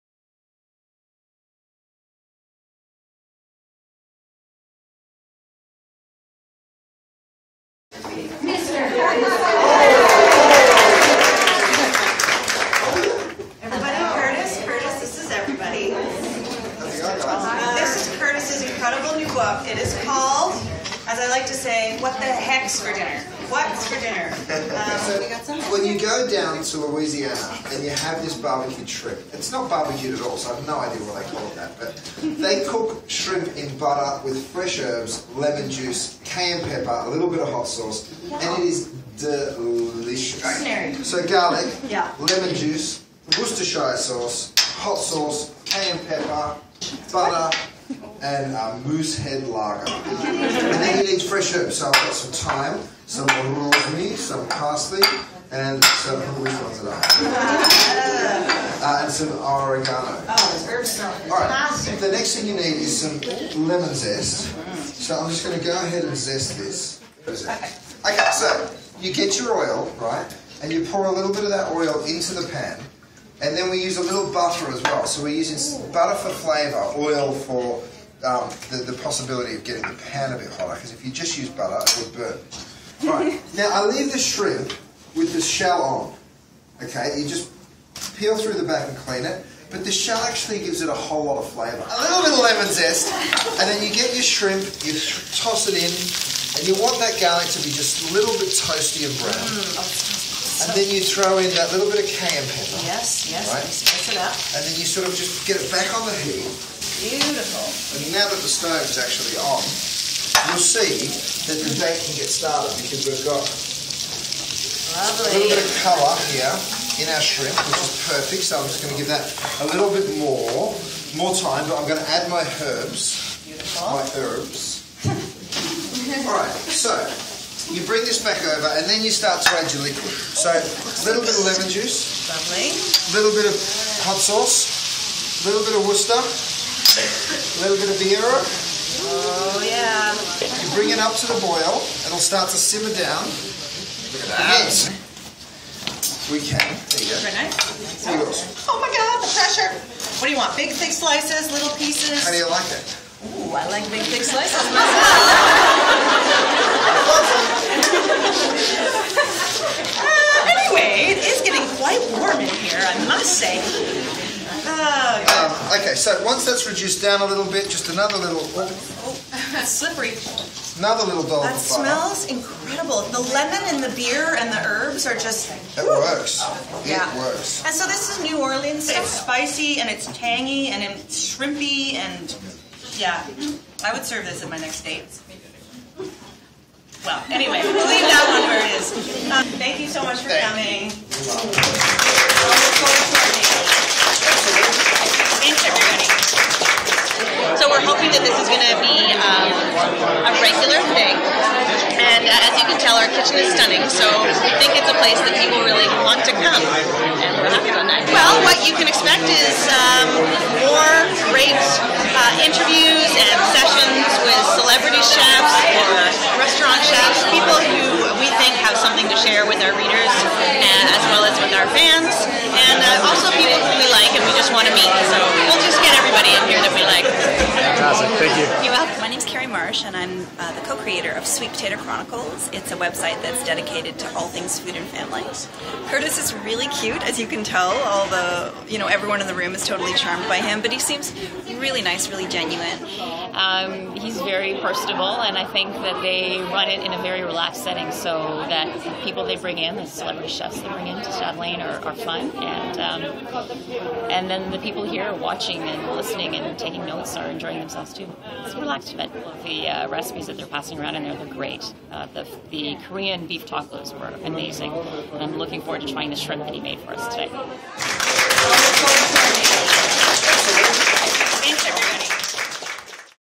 Curtis Stone Heats up the Chatelaine Kitchen
Curtis Stone got things cooking at the Chatelaine Kitchen with recipes from his new book, What's For Dinner?...